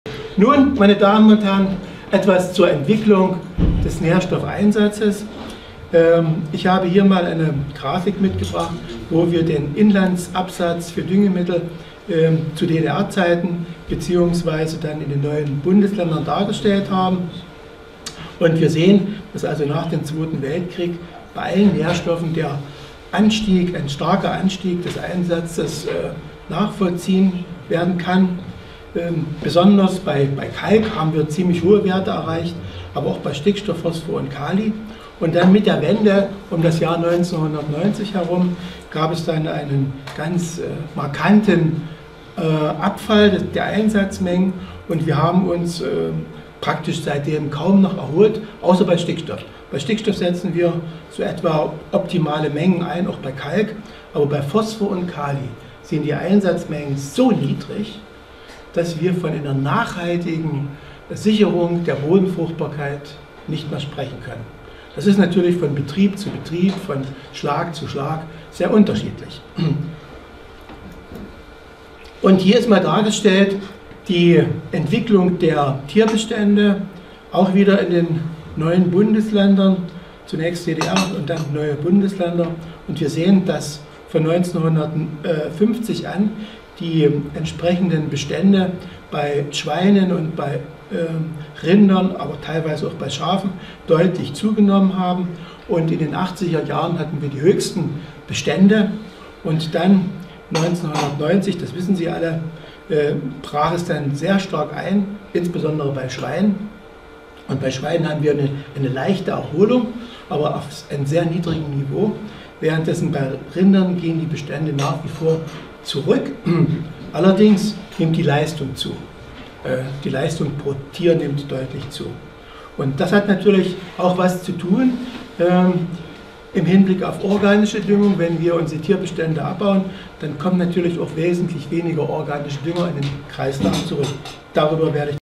Dr. E. Albert - Teil 3: Nährstoffeinsatz; Seminar des VFuD - " Angewandte Pflanzenernährung "
Teil 3: Entwicklung des Nährstoffeinsatzes in der Landwirtschaft seit 1930 + Die Entwicklung der Tierbestände + Die Entwicklung der organischen Düngung
Am 29. November 2012 fand in Friedefeld (bei Penkun, an der A 11, kurz vor Stettin) das fünfte Praxisseminmar des Jahres 2012 des VFuD (Verein zur Förderung einer umweltschonenden Düngung) statt. Als Referent war der bekannte Agrarwissenschaftler Dr. Erhard Albert (aus Bautzen) eingeladen.
Es war ein besonders intensiver und sehr interessanter Vortrag.
Alberts Fach-Beitrag für den VFuD dauerte an diesem Tag mehr als zwei Stunden...
Deshalb haben wir dieses "geballte" praktische Fachwissen für unsere (VFuD-) Mitglieder nach den angesprochenen Themen zerschnitten.
So entstanden aus dem Gesamtvortrag 9 Themenbeiträge.
Hier finden sie den dritten Teil -  Wie hat sich der Nährstoffeinsatz von 1930 bis heute entwickelt?